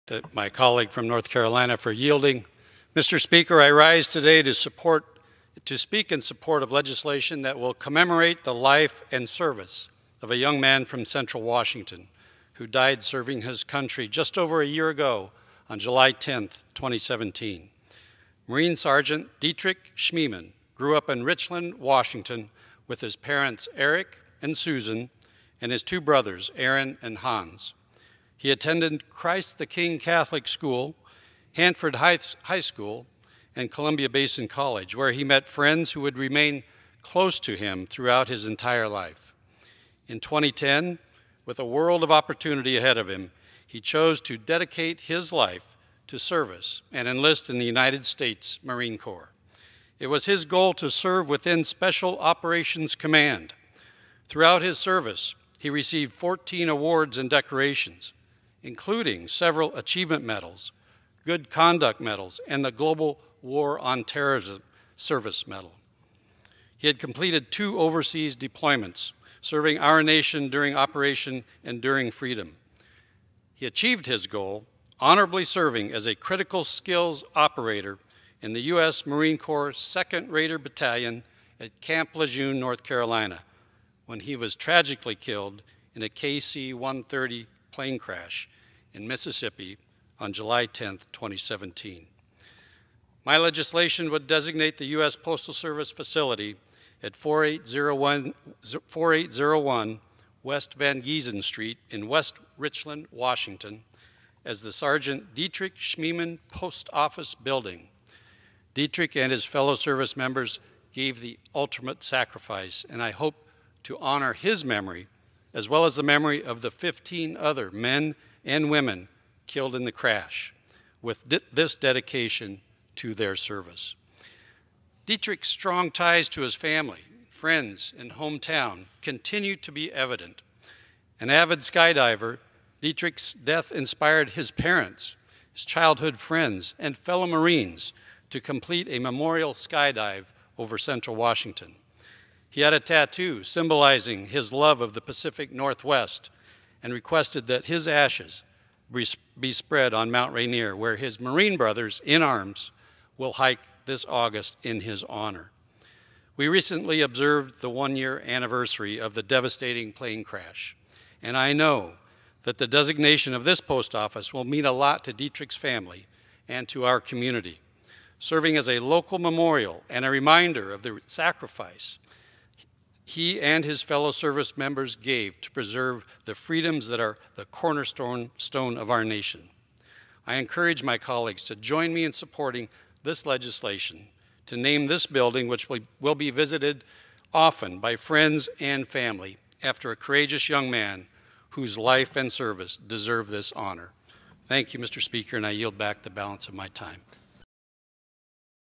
Newhouse Speaks in Support of Legislation to Honor Fallen Marine
7/16/2018 Rep. Newhouse (R-WA) speaks in support of his legislation, H.R. 5504, which would designate a U.S. Postal Service facility in West Richland, Washington as the "Sergeant Dietrich Schmieman Post Office Building."